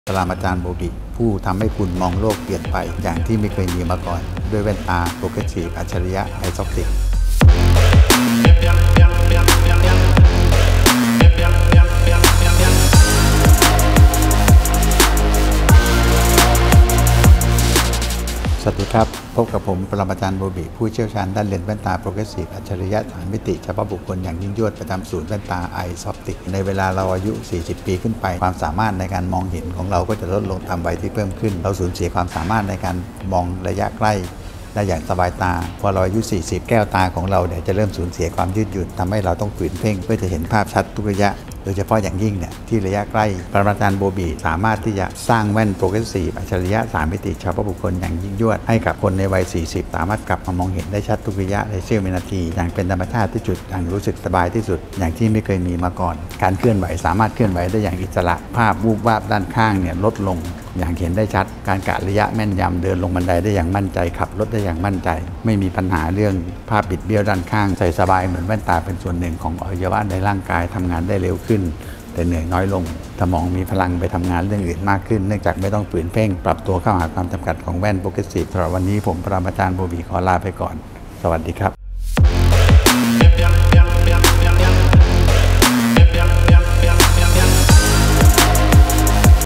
ปรมาจารย์โบบิ ผู้เปลี่ยนมุมมอง ให้คุณได้มองโลกเปลี่ยนไปด้วยเลนส์โปรเกรสซีฟอัจฉริยะไอซอพติก
เวลาเราอายุ 40 ปีขึ้นไป ความสามารถในการมองเห็นของเราก็จะลดลงตามวัยที่เพิ่มขึ้น เราสูญเสียความสามารถในการมองระยะใกล้ได้อย่างสบายตา พอเราอายุ 40 ปี แก้วตาของเราจะเริ่มสูญเสียความยืดหยุ่น ทำให้เราต้องฝืนเพ่ง เพื่อจะเห็นภาพขัดทุกระยะ โดยเฉพาะอย่างยิ่งที่ระยะใกล้

ปรมาจารย์โบบิ สามารถที่จะสร้างแว่นโปรเกรสซีฟอัจฉริยะ 3 มิติ เฉพาะบุคคลอย่างยิ่งยวด ให้กับคนในวัย 40 ปี สามารถกลับมามองเห็นได้ชัดทุกระยะในเสี้ยววินาที อย่างเป็นธรรมชาติที่สุด อย่างรู้สึกสบายที่สุด อย่างที่ไม่เคยมีมาก่อน การเคลื่อนไหวสามารถเคลื่อนไหวได้อย่างอิสระ ภาพวูบวาบด้านข้างลดลงอย่างเห็นได้ชัด การกะระยะแม่นยำ เดินลงบันไดได้อย่างมั่นใจ ขับรถได้อย่างมั่นใจ ไม่มีปัญหาเรื่องภาพบิดเบี้ยวด้านข้าง ใส่สบายเหมือนแว่นตาเป็นส่วนหนึ่งของอวัยวะในร่างกาย ทำงานได้เร็วขึ้น แต่เหนื่อยน้อยลง สมองมีพลังไปทำงานเรื่องอื่นมากขึ้น เนื่องจากไม่ต้องฝืนเพ่งปรับตัวเข้าหาความจำกัดของแว่นโปรเกรสซีฟ

สายด่วน ปรมาจารย์โบบิ : ผู้เชี่ยวชาญด้านแว่นโปรเกรสซีฟอัจฉริยะ 3 มิติ เฉพาะบุคคลอย่างยิ่งยวด